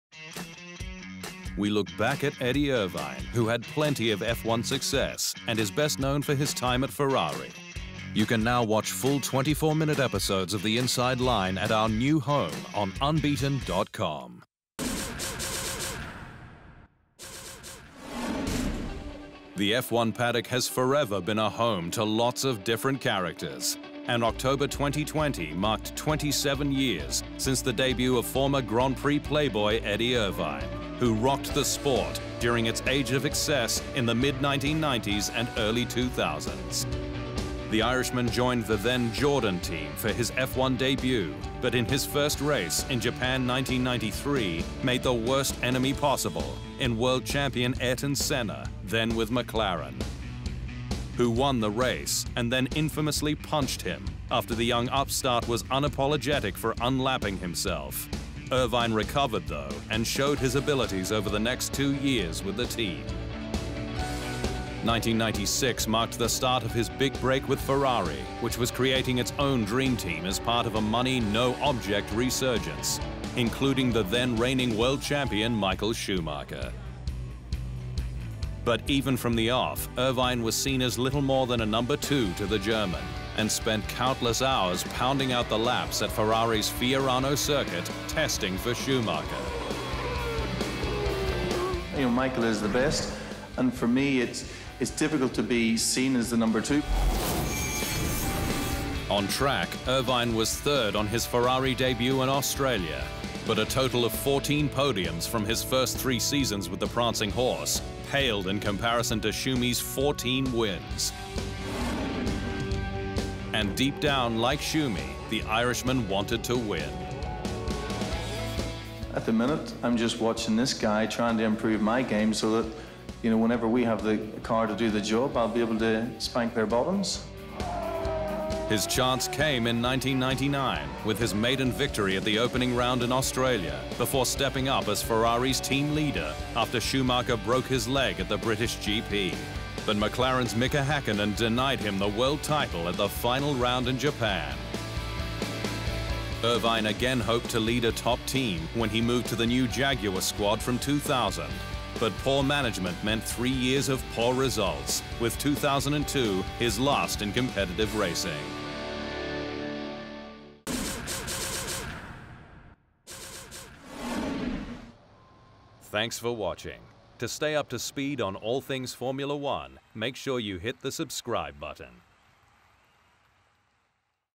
F1 RETRO: 1990s/2000s racing star Eddie Irvine
The F1 paddock has forever been a home to lots of different characters, and October 2020 marked 27 years since the debut of former Grand Prix playboy Eddie Irvine, who rocked the sport during its age of excess in the mid-1990s and early 2000s.

The Irishman joined the then-Jordan team for his F1 debut, but in his first race, in Japan 1993, made the worst enemy possible, in world champion Ayrton Senna - then with McLaren.

Follow @unbeatenf1 on Facebook, Twitter and Instagram.